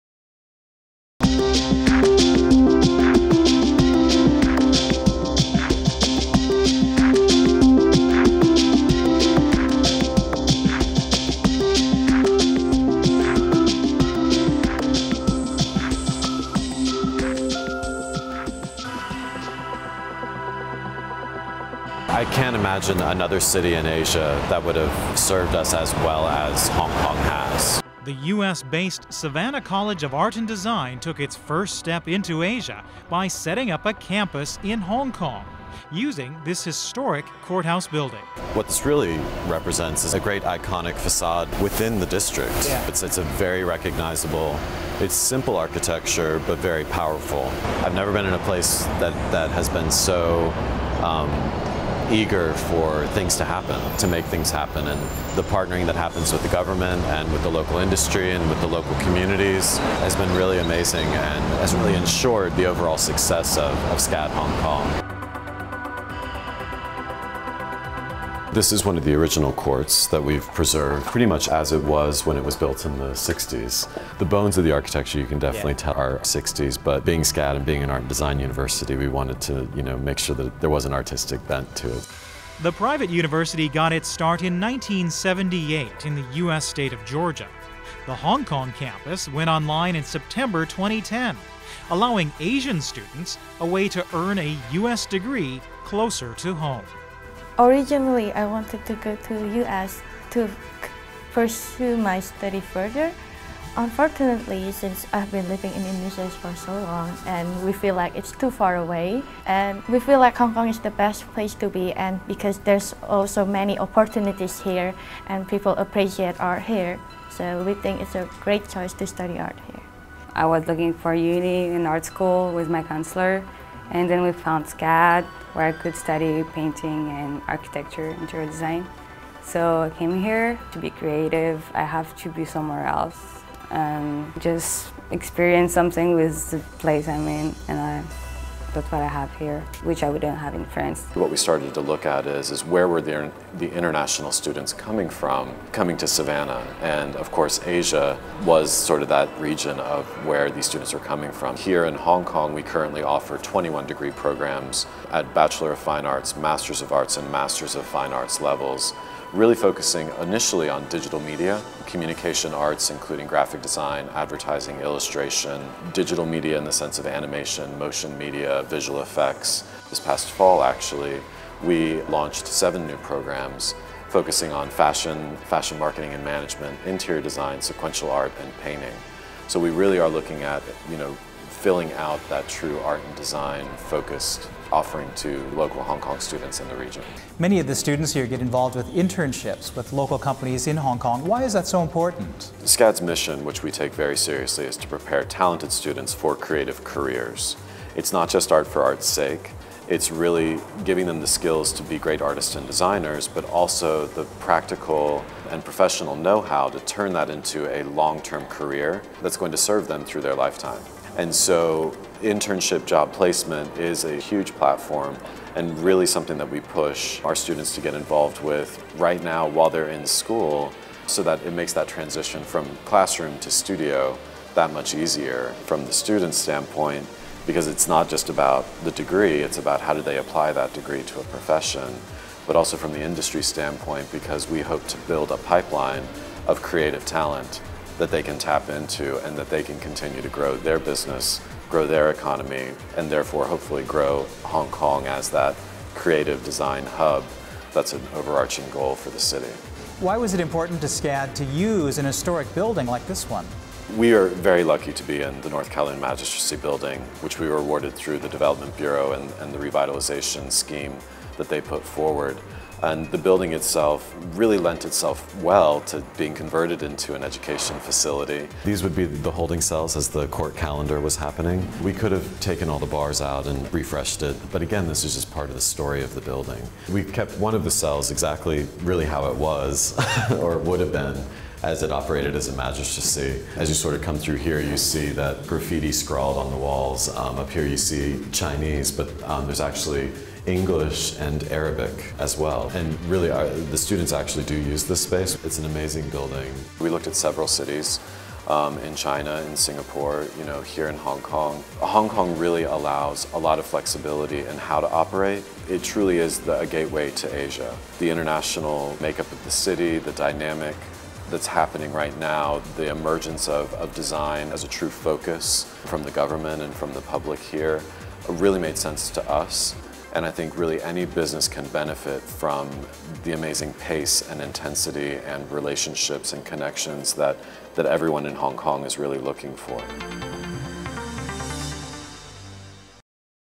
Smart Choice: US Design School SCAD in Hong Kong
American private university Savannah College of Art and Design (SCAD) has for nearly three years offered internationally recognized courses in Hong Kong at a campus set up in an historic courthouse. SCAD Hong Kong's Associate Vice President Grant Preisser gave the HKTDC a tour as he explained why the city was a smart choice for the institution's first Asian campus.

Speakers:
Grant Preisser    Associate Vice President
SCAD Hong Kong

Novita Permatasari
SCAD Hong Kong Student